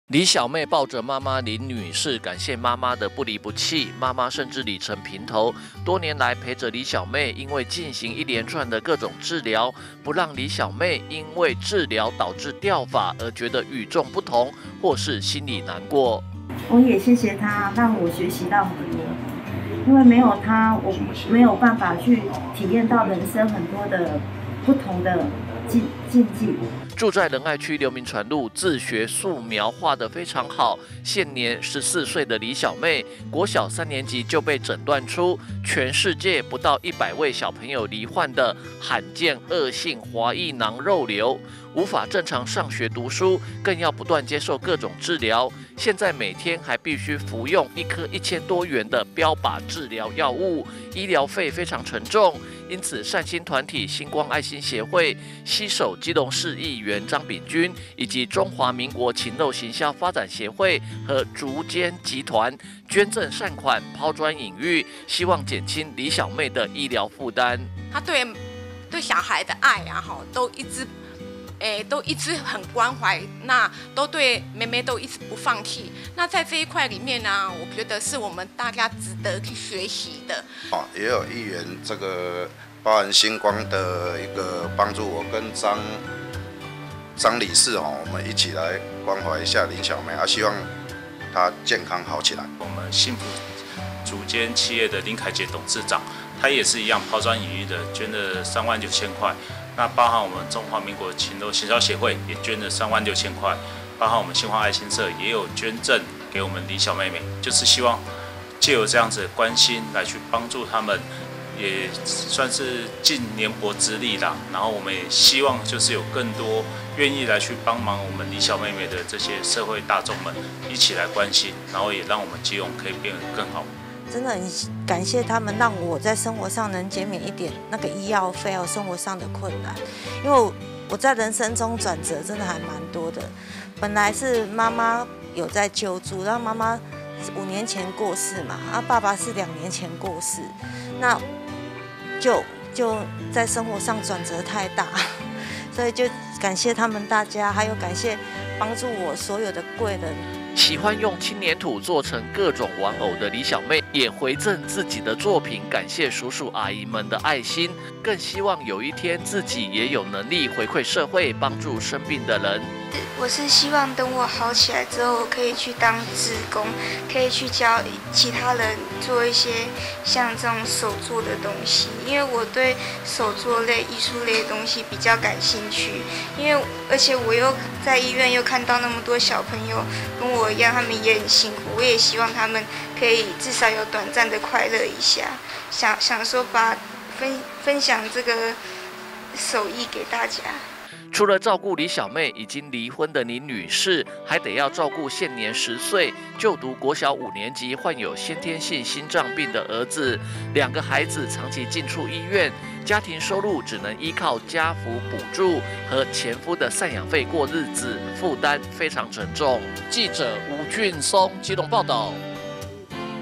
基隆|張秉鈞|罕病|惡性滑液囊肉瘤|星光愛心協會|中華民國禽肉行銷發展協會|築間集團|善款|醫療費|治療|癌症|劉銘傳路|素描|輕黏土作品|生命鬥士|善心團體攜手張秉鈞 捐贈善款幫助罕病李小妹
善心團體攜手張秉鈞 捐贈善款幫助罕病李小妹

(中嘉吉隆新聞記者吳俊松/基隆報導)住在仁愛區劉銘傳路，現年14歲的李小妹，國小三年級就被診斷出全世界不到一百位小朋友罹患的罕見惡性「滑液囊肉瘤」，無法正常上學讀書，更要不斷接受各種治療，現在每天還必須服用一顆一千多元的標靶治療藥物，醫藥費非常沉重，因此善心團體星光愛心協會，攜手基隆市議員張秉鈞，以及中華民國禽肉行銷發展協會和築間集團，捐贈善款，拋磚引玉，希望減輕李小妹的醫療負擔！

李小妹抱著媽媽林女士，感謝媽媽的不離不棄，媽媽甚至理成平頭，多年來陪著李小妹因為進行一連串的各種治療，不讓李小妹因為治療導致掉髮而覺得與眾不同或心裡難受。

住在仁愛區劉銘傳路，自學素描畫得非常好，現年14歲的李小妹，國小三年級就被診斷出全世界不到一百位小朋友罹患的，罕見惡性「滑液囊肉瘤」，無法正常上學讀書，更要不斷接受各種治療，現在每天還必須服用一顆一千多元的標靶治療藥物，醫藥費非常沉重，因此善心團體星光愛心協會，攜手基隆市議員張秉鈞，以及中華民國禽肉行銷發展協會和築間集團，捐贈善款，拋磚引玉，希望減輕李小妹的醫療負擔！

喜歡用輕黏土做成各種玩偶的李小妹，也回贈自己的作品感謝叔叔阿姨們的愛心，更希望有一天自己也有能力回饋社會，幫助生病的人！

除了照顧李小妹，已經離婚的林女士，還得要照顧現年10歲，就讀國小五年級患有先天性心臟病的兒子，兩個孩子長期進出醫院，家庭收入只能依靠家扶補助和前夫的贍養費過日子，負擔非常沉重！